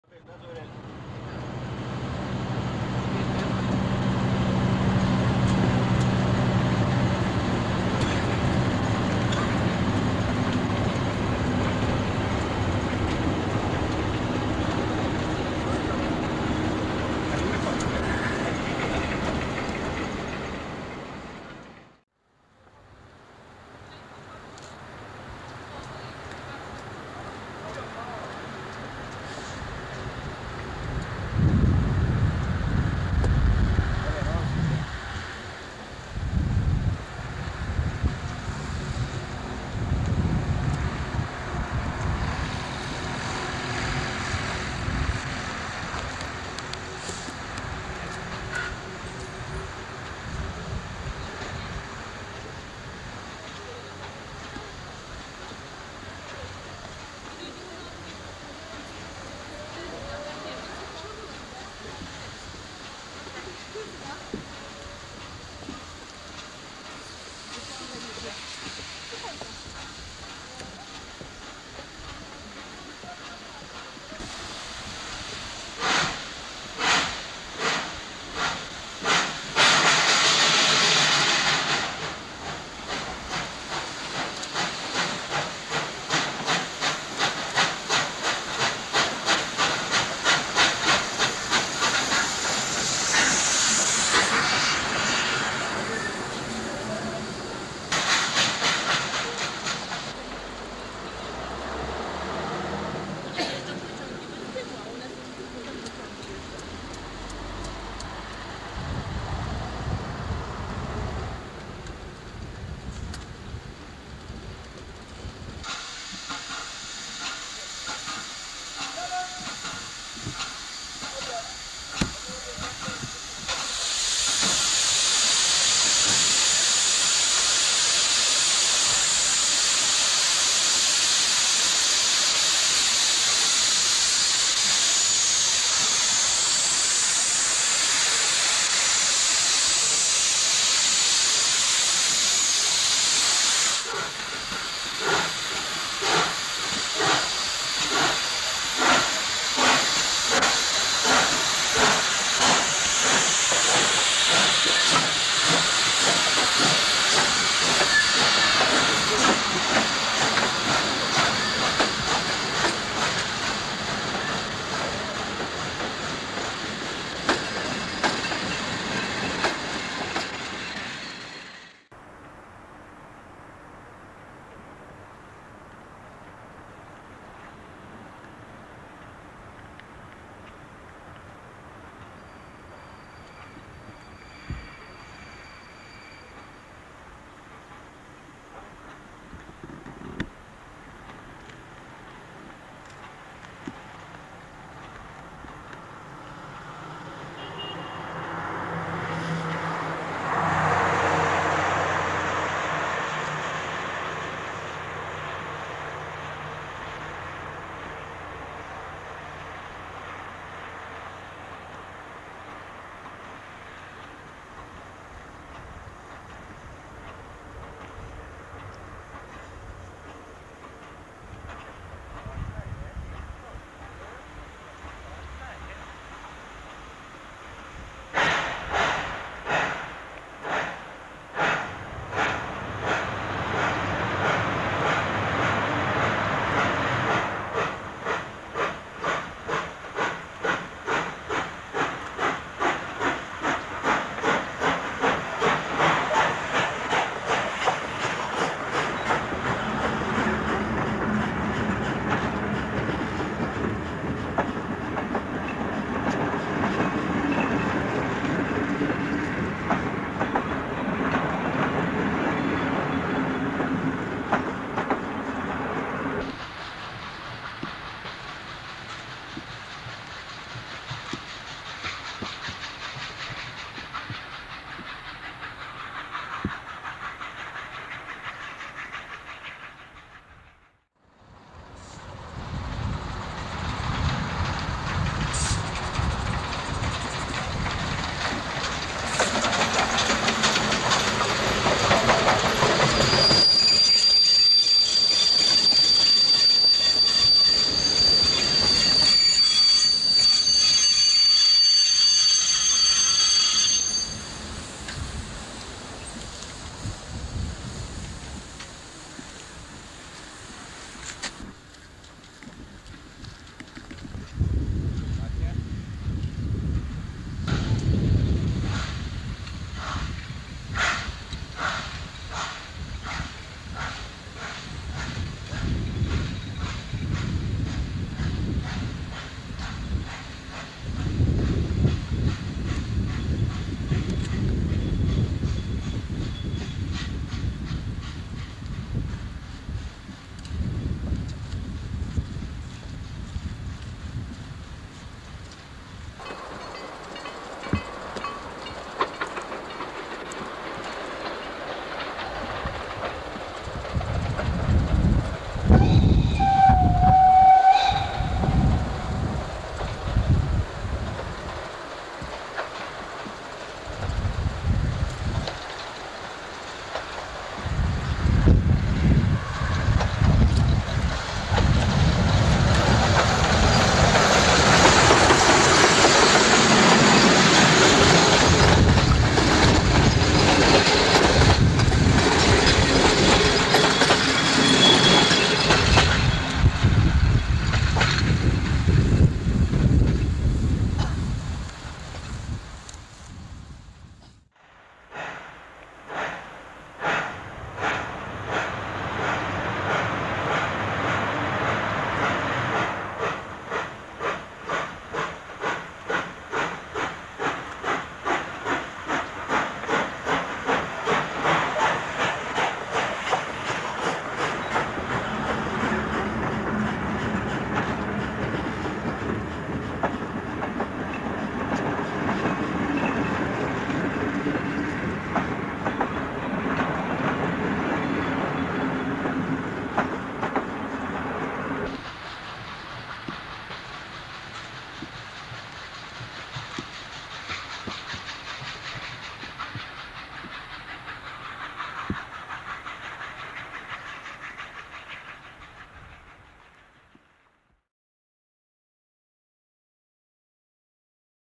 Steamrailway. With Ol49 no 4 to Ruciane Nida in 1991.
1991 was the last year with a lot of steam locomotive traffic on the steam railways around Elk in eastern Poland.
We experienced steam locos on the line to Mikolajki/ Mragowo. Ruciane Nida. Goldap, Suwalki and some sidelines in direction of Korze.
Locomotives class Ty2 / Ty42 and Ol49.
This video have scenes from the line to Ruciane Nida, with the loco class Ol49 no 4. with a train from Elk to Ruciane Nida and back.
I had a hired VHS camera, so big that i had to  rest it on my shoulder. (!)
The quality of the VHS videos was not good and the camera man was a novice but i hope You like it anyway!
It is of great help to reach new wiewers.